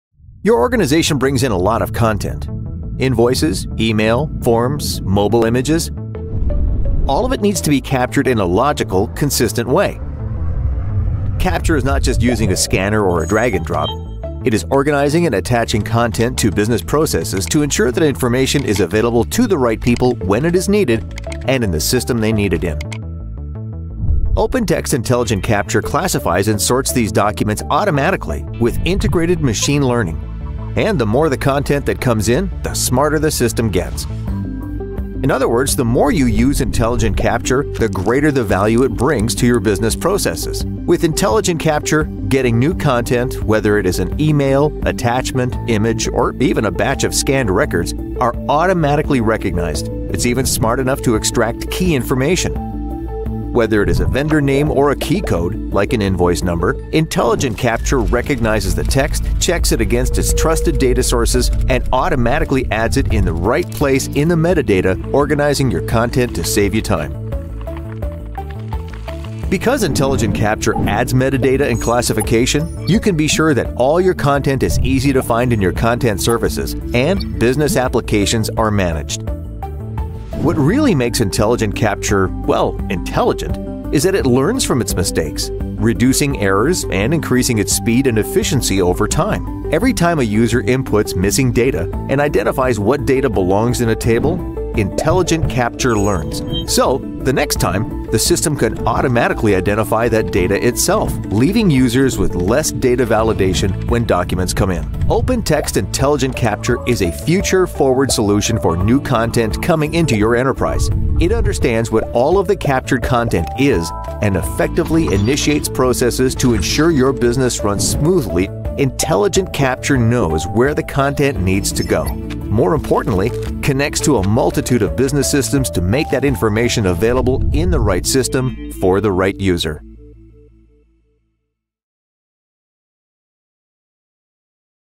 OpenText Intelligent Capture (Formerly Captiva) Overview
Learn how OpenText Intelligent Capture, formerly OpenText Captiva, serves as enterprise capture platform, providing omni-channel capabilities for collecting everything from scanned paper to chatbots. It is not just for organizing content at the front door, but automating processes across the enterprise. Intelligent Capture can automate processes for standard documents, such as accounts payable invoice processing, new customer and loan onboarding, accounts receivable, case management, contract management, and much more.